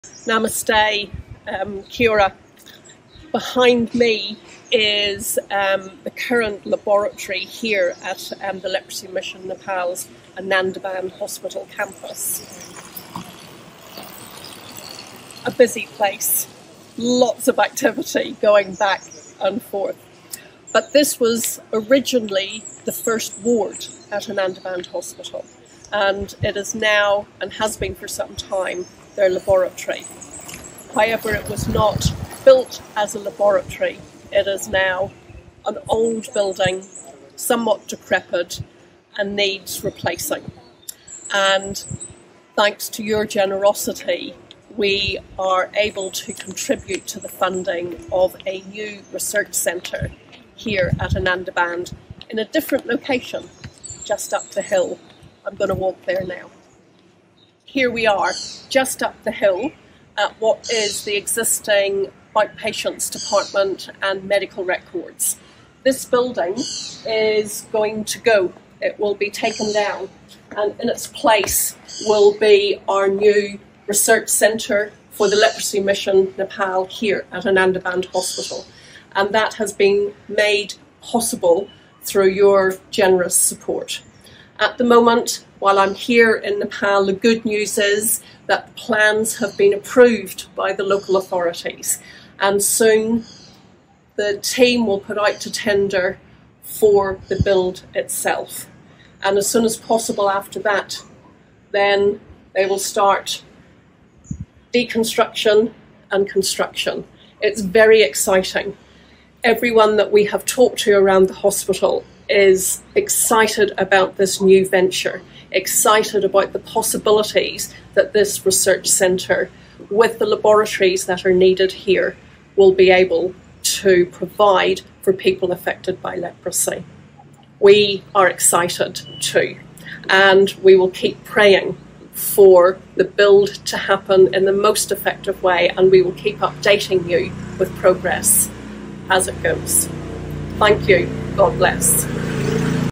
New Research Centre in Nepal | Anandaban Hospital
Gillian (Leprosy Mission NZ Executive Director) was in Nepal earlier this year. She visited Anandaban Hospital and wanted to update you on the progress of the new research centre.

Anandaban Hospital is the largest hospital in Nepal specialising in leprosy treatment. Its staff team carry out world-leading research to improve understanding of the disease and develop new treatments.

The new Research Centre will allow its team of scientists and clinicians to operate at their most efficient.